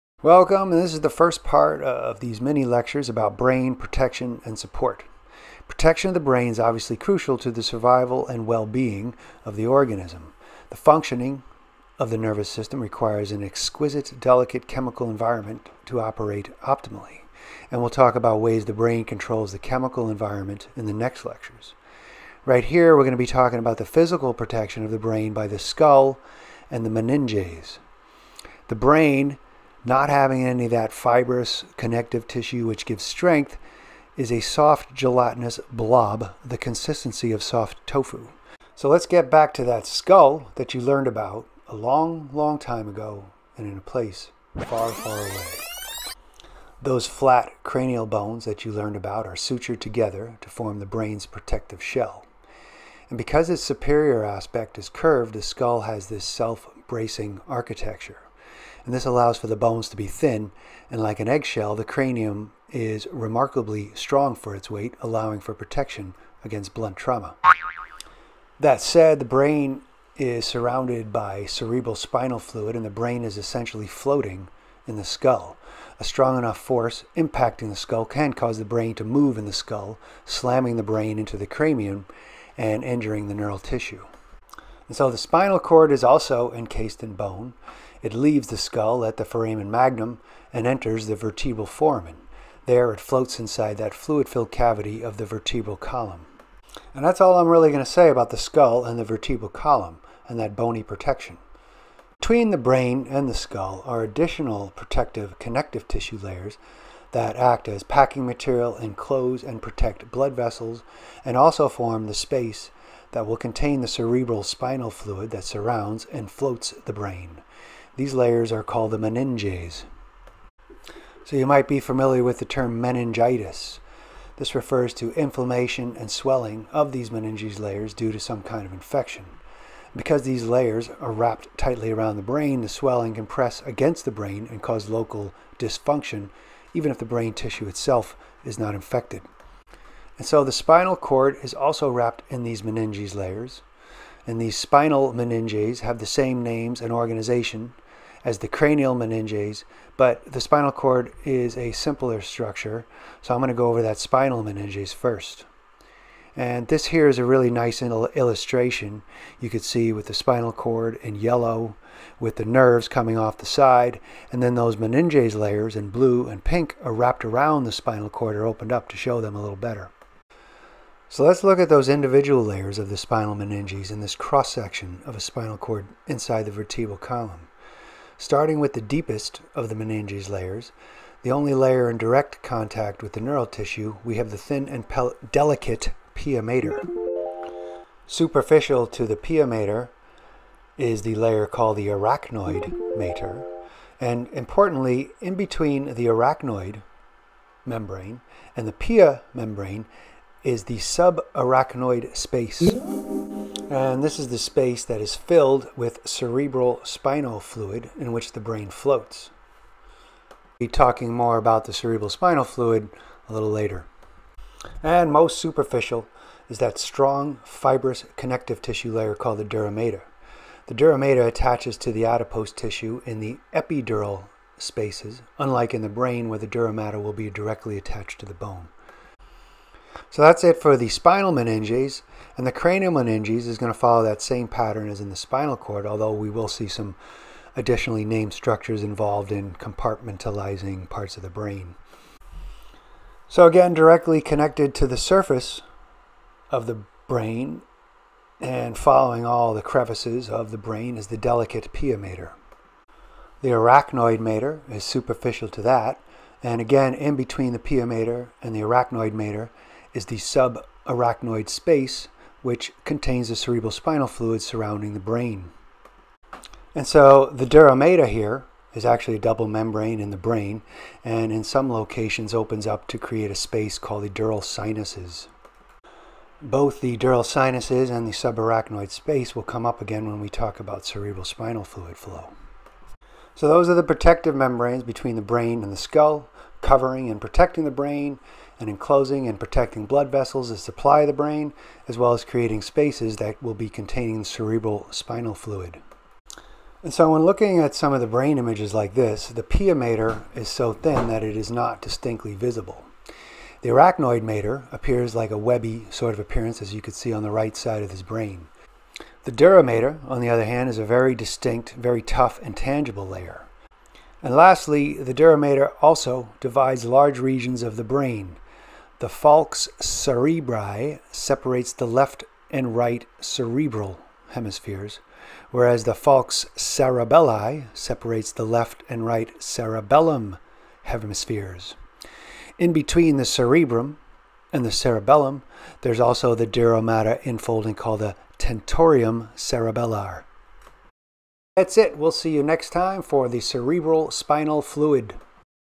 L13.3A Brain Protection
This covers how the brain is physically protected by the skull, vertebral column and the meninge layers